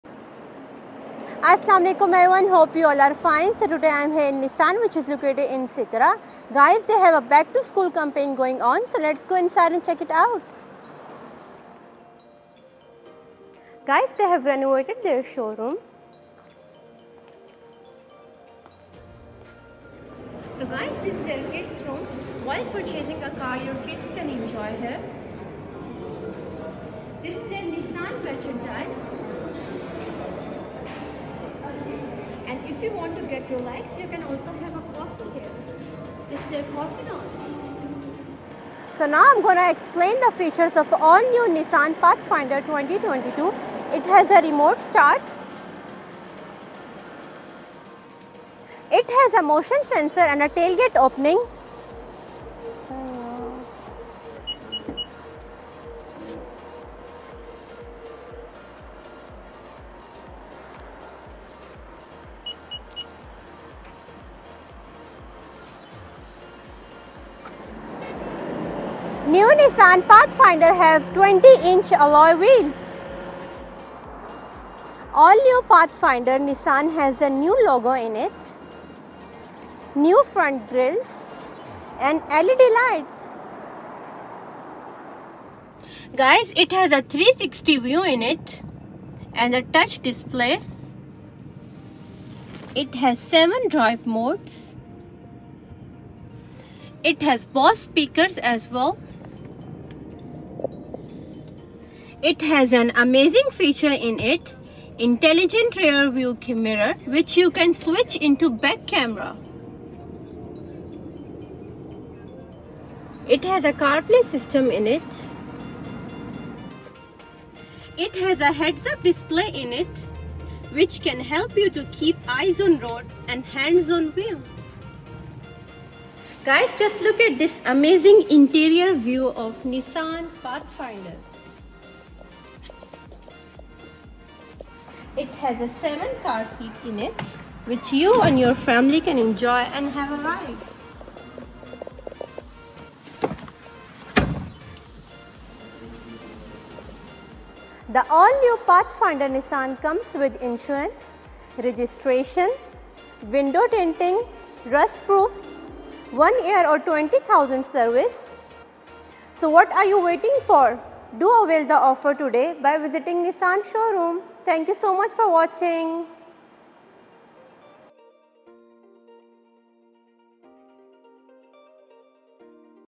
Nissan Bahrain 🇧🇭 | Pathfinder 2022 | Car , Jeep | Mahwish Awais
Nissan Bahrain
Is located in Sitra

⚫️They have back to School Campaign going ok

⚫️They have renovated there showroom

⚫️They have new Nissan pathfinder with advanced features

⚫️At Nissan, our technologically advanced options include
-VAT
- One year free service or 20,000km
-Comprehensive 5years Nissan warranty
-Rust proofing
-Window Tinting
-Insurance
-Registration

17732732 / 37112112